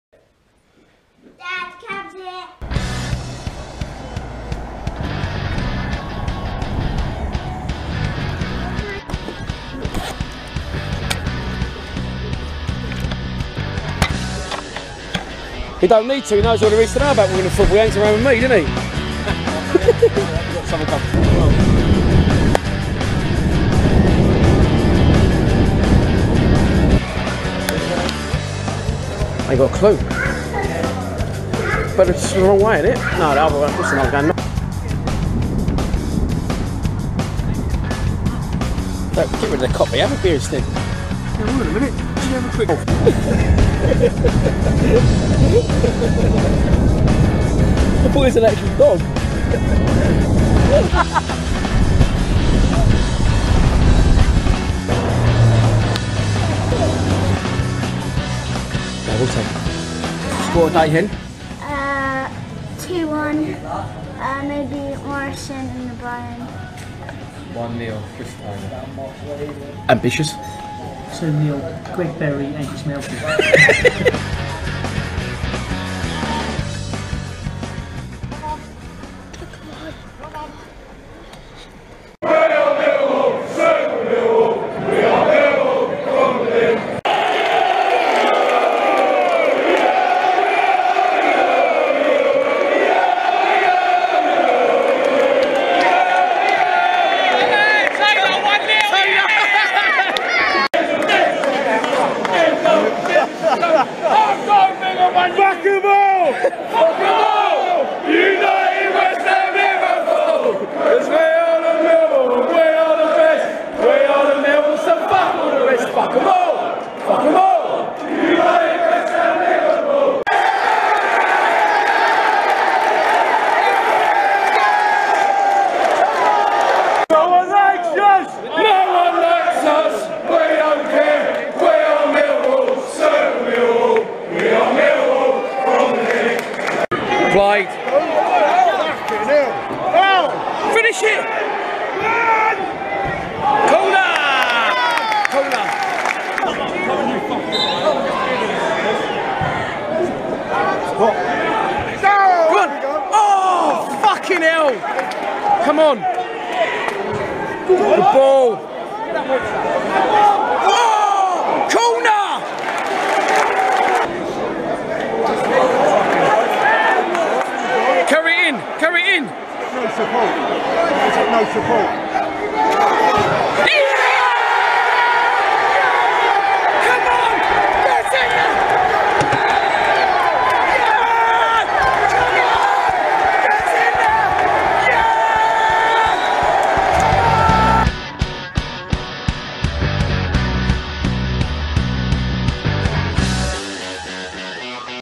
Away day 2 - Bury - Buried!!
Lions TV's trip to Bury - A 3-1 win, match predictions, a man with a horse's head, and singing from Denzil.

COYL!